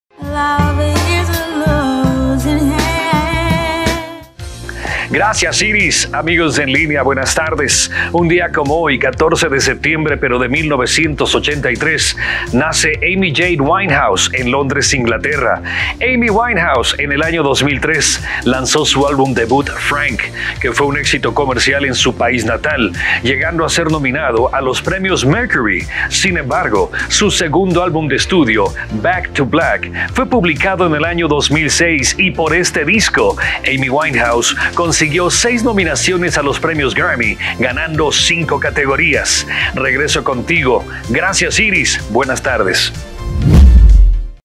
Amy Winehouse, un poco de su historia.
Amy Winehouse nació un día como hoy, pero de 1983. Gracias a su segundo disco obtuvo 6 nominaciones en los premios Grammy en 2006, y 6 años después entró a la lista de "Las 100 mejores mujeres en la música". Sin duda, una mujer exitosa pese a su comportamiento autodestructivo.